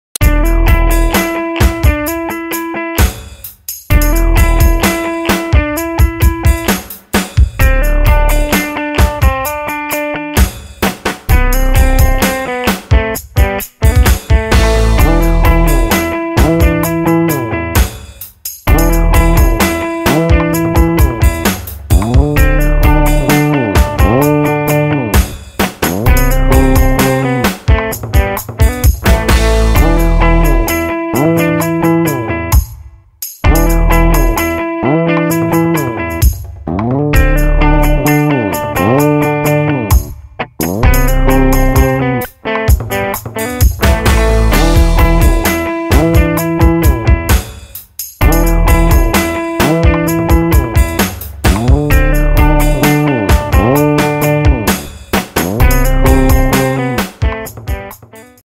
Simplicity & Smoothness - Card Magic Trick [2]
Finally something new about my magic videos!
This is just a simple handling video, nothing new.